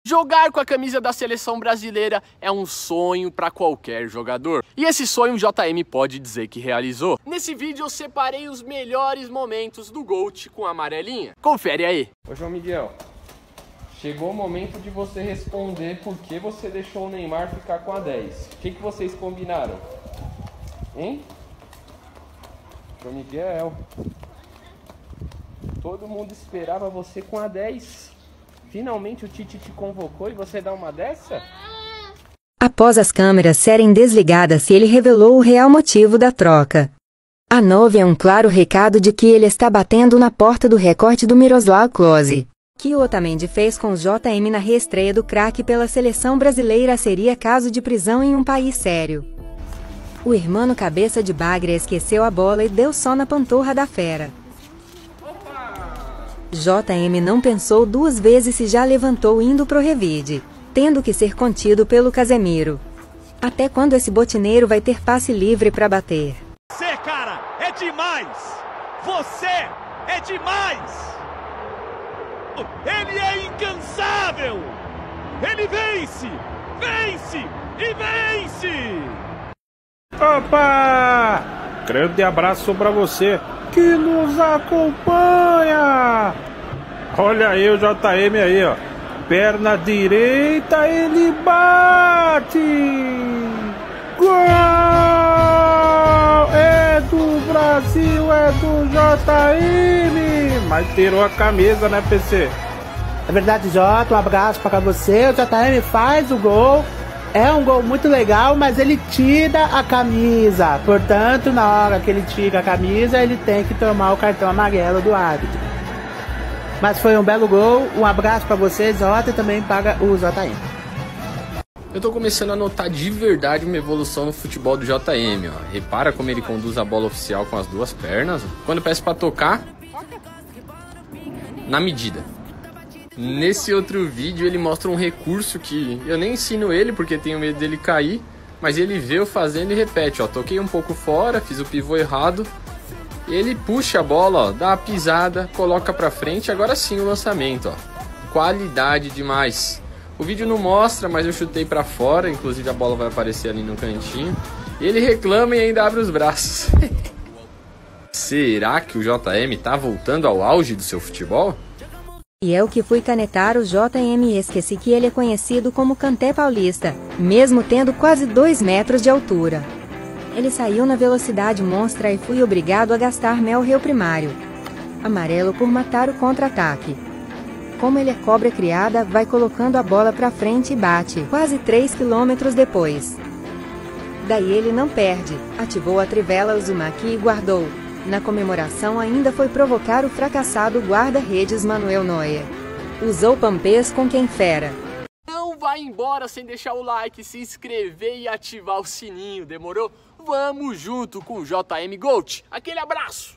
MELHORES MOMENTOS DO JM NA SELEÇÃO BRASILEIRA | JM GOAT
JM está resgatando o amor do povo pela Seleção, isso é bonito!
JM GOAT: o pesadelo do Messi e do CR7

🔔 Lacerdovisk (pai e empresário do JM):
✓ Instagram e TikTok →  @lacerdovisk